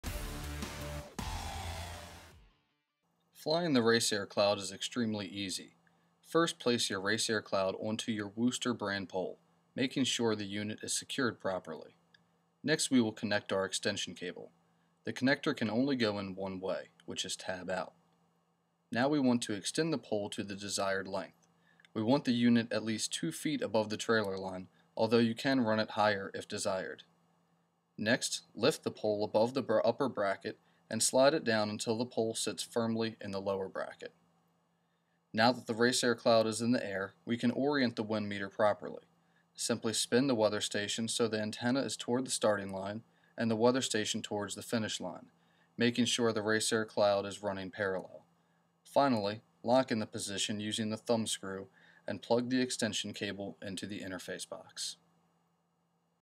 RaceAir Cloud - Flying The RaceAir Cloud
Introducing the new Computech RaceAir Cloud Racing Weather Station. Designed by racers for racers, the RaceAir Cloud has all the great weather variables as before but now offers Wind Speed, Wind Direction, Cloud Cover and Texting capability